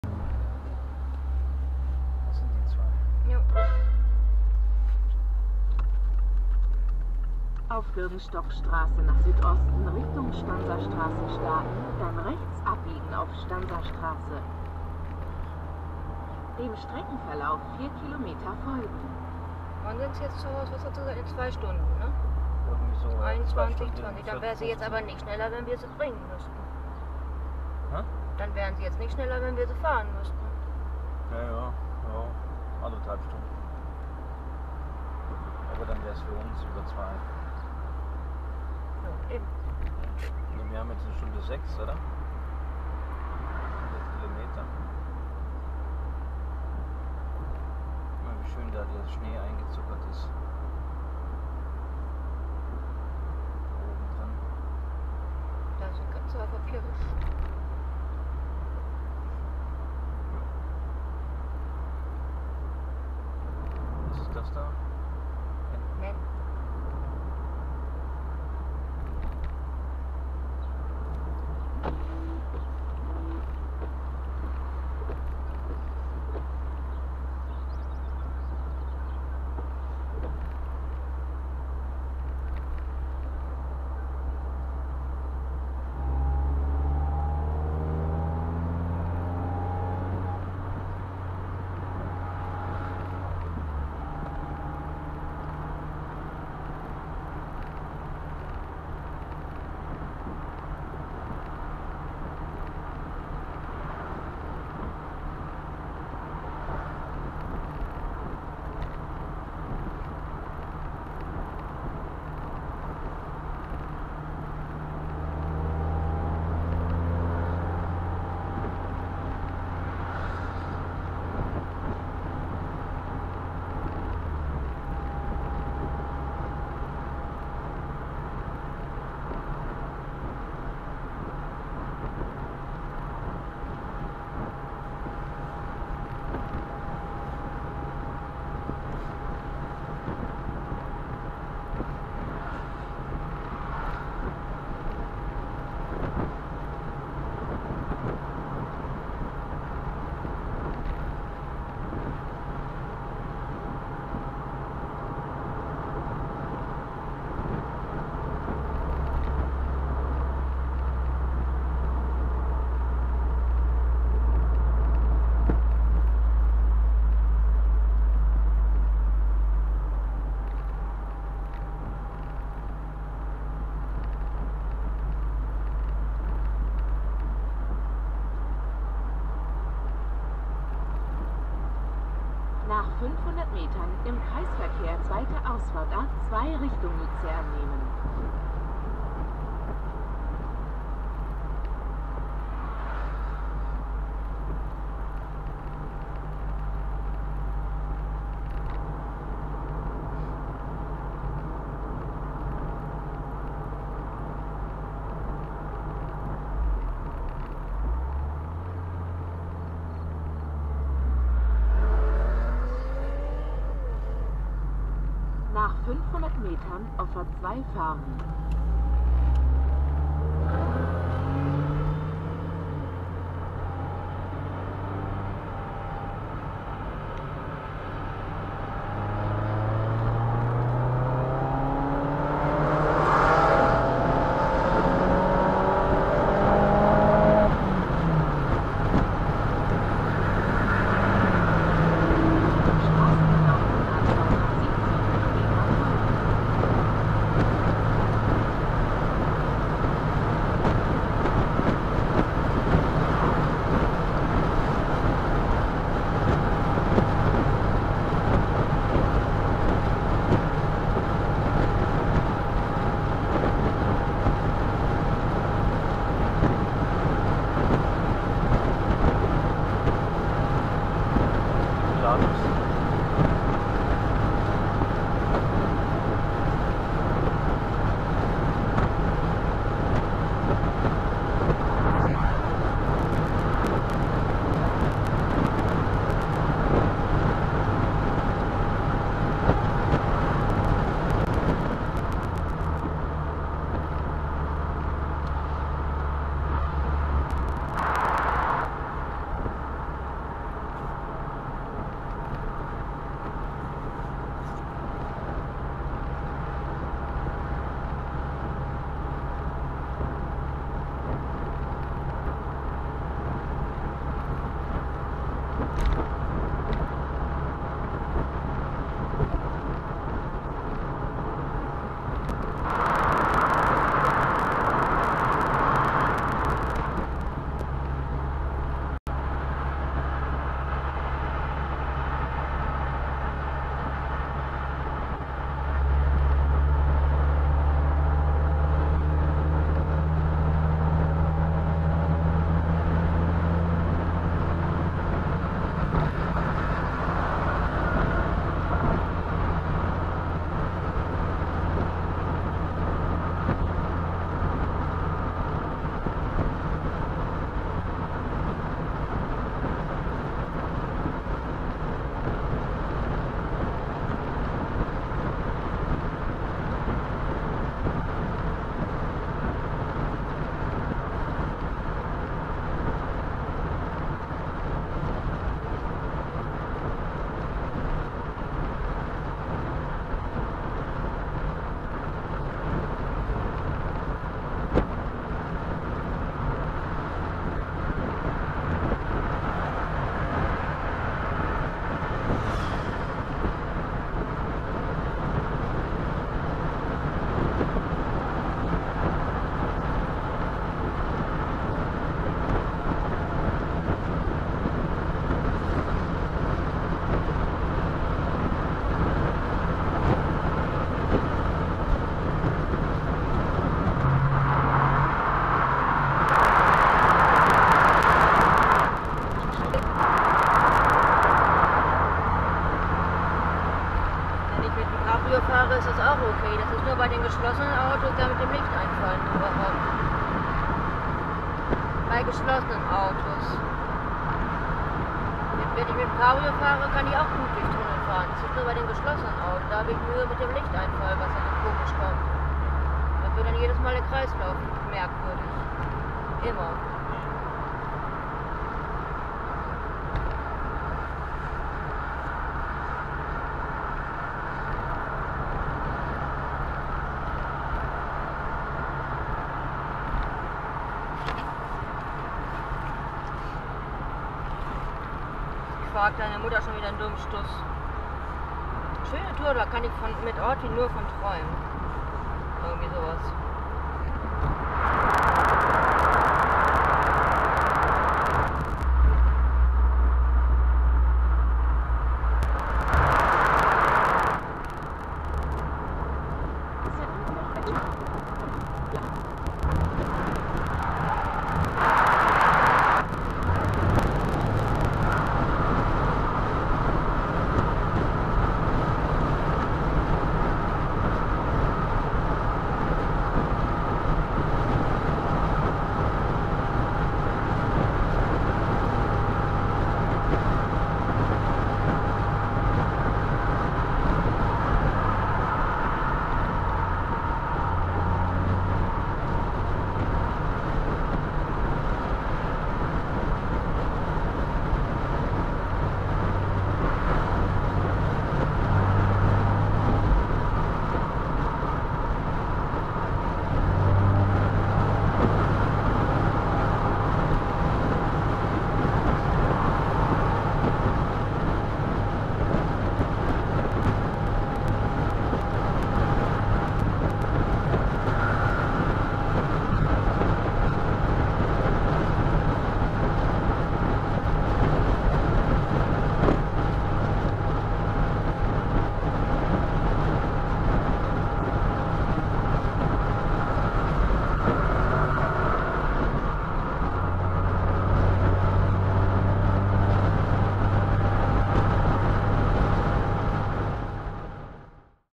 Ächerli 5 - Out of the Mountains
Nichts ist melancholischer, als nach einen schönen Tag die Berge in der Abendstimmung wieder zu verlassen. Wie ein Magnet versuchen sie einen fest zu halten, nur langsam lässt ihre anziehende Wirkung nach, wie die warme Abendsonne sich zwischen uns und sie schiebt.